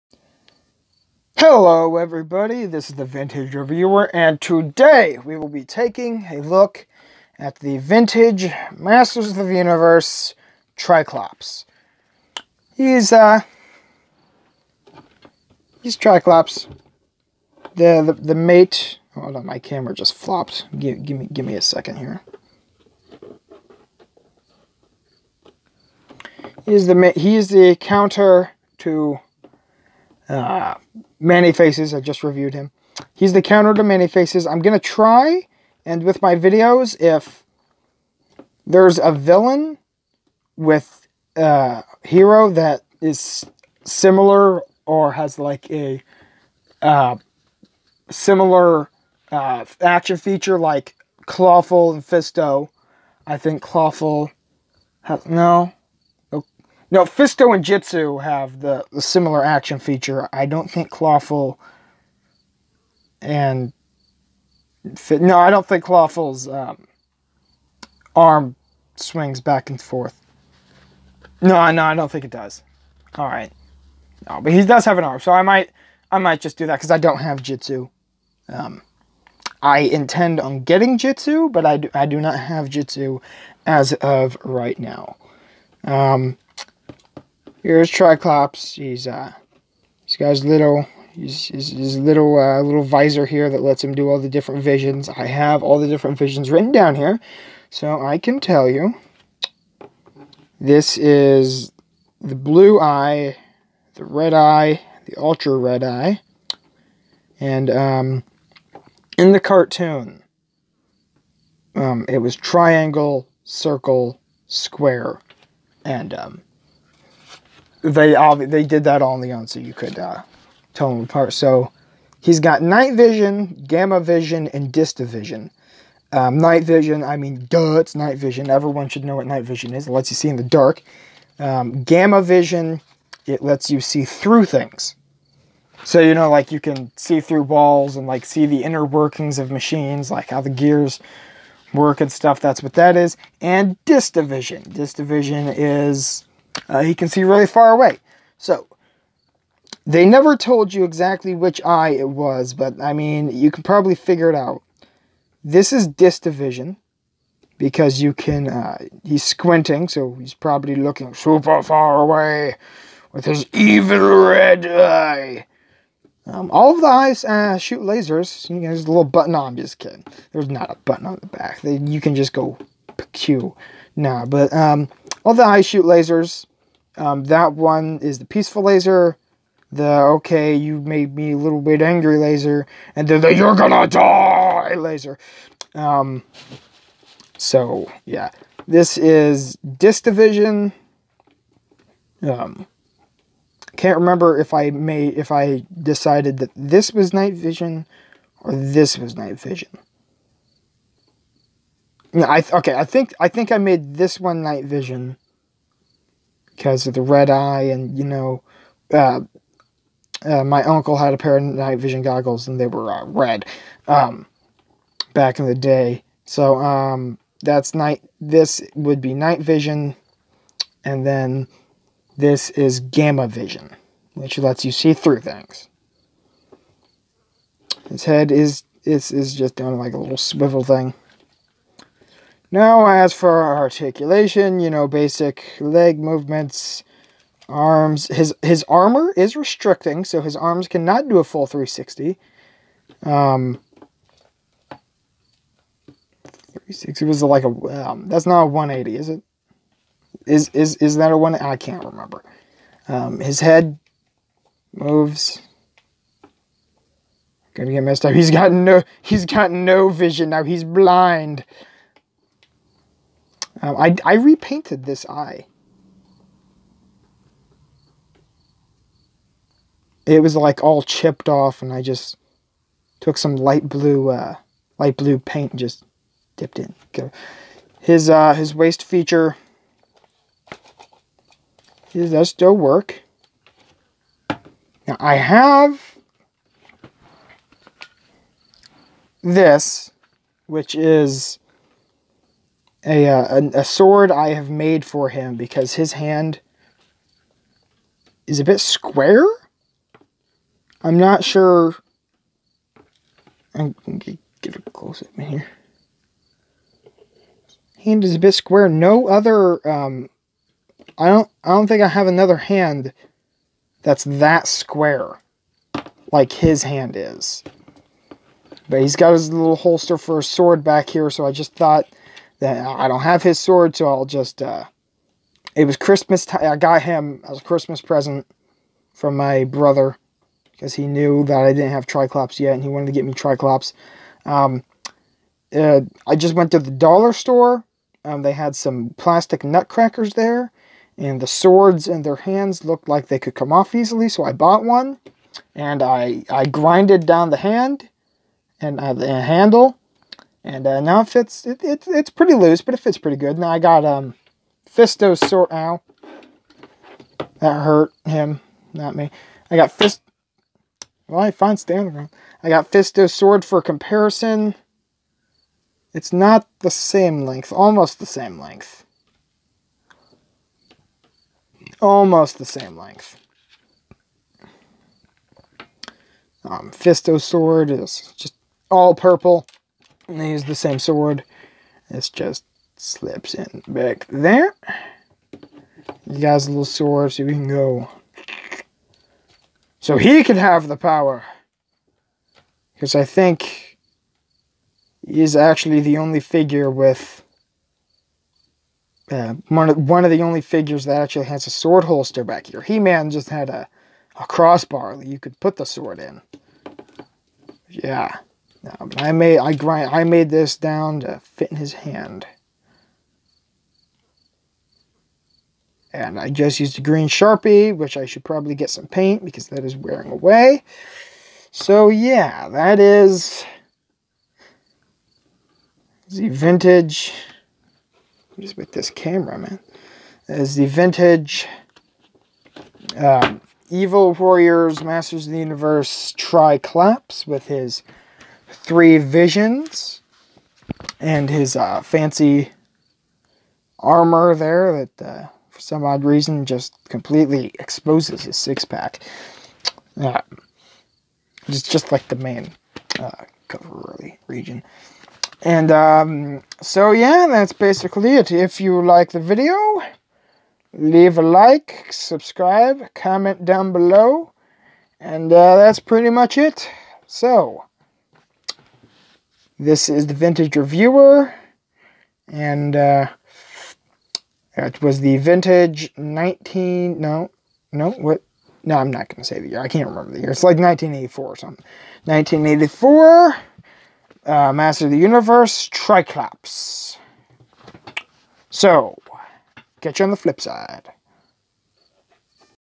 Motu Triclops review
while some have three different personalities few have three different eyes,watch as I review Masters of the Universe triclops